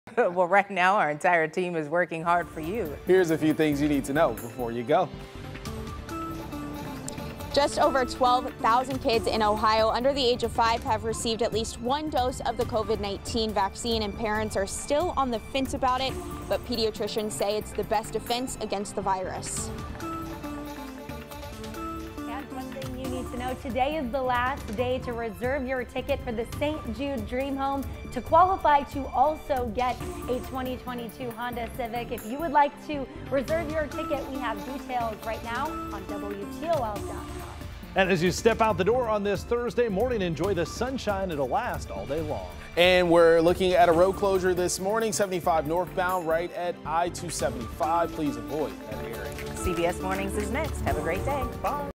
Things you need to know before you go: 7 a.m.
Madelyne Watkins, Amy Steigerwald, Ryan Wichman, Steven Jackson and Tiffany Tarpley share some things you need to know before you go.

Over 12,000 children under 5 years old in Ohio have received at least one dose of the COVID-19 vaccine, today at 11:59 p.m. is the last chance to win a 2022 Honda Civic by reserving your ticket for the St. Jude Dream Home, today's sunshine will last all day long, part of I-75 northbound by I-275 is closed following a crash.